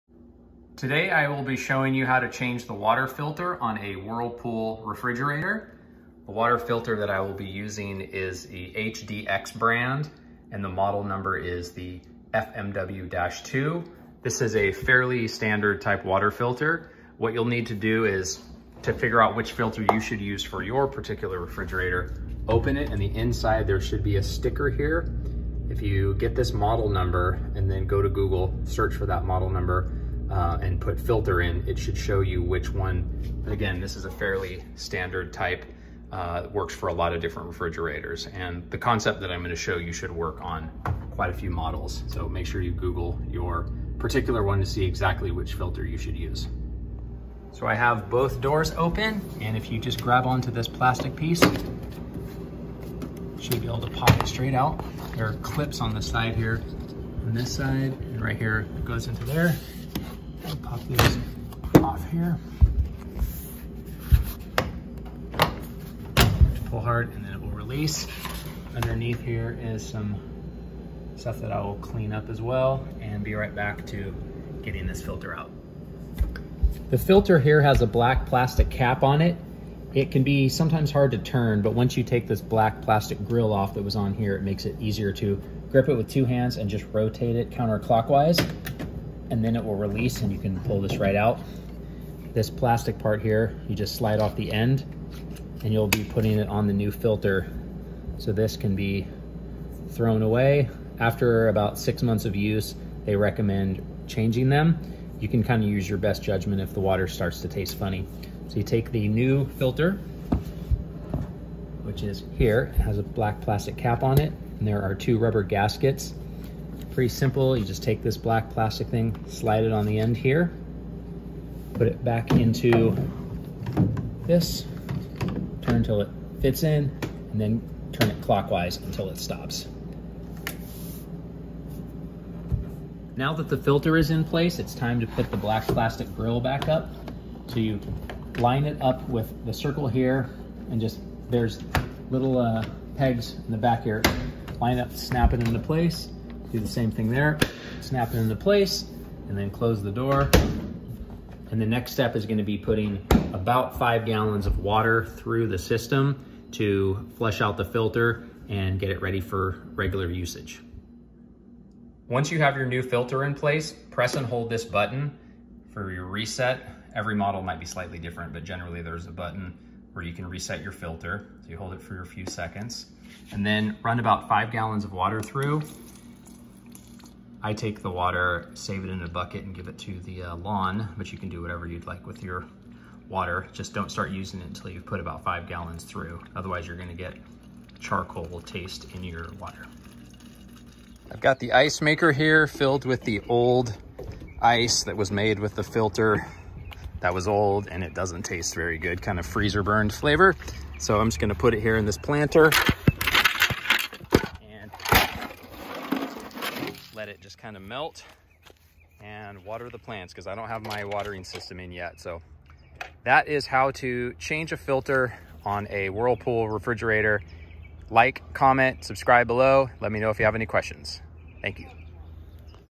How to Replace the Water Filter on a Whirlpool Gold Side-By-Side Refrigerator
I show you the step by step guide to replacing the FMW-2 water filter cartridge on my Whirlpool Gold GS6SSHEXNL04 side by side refrigerator. Many refrigerators use this same type of system so this video may be of help to you!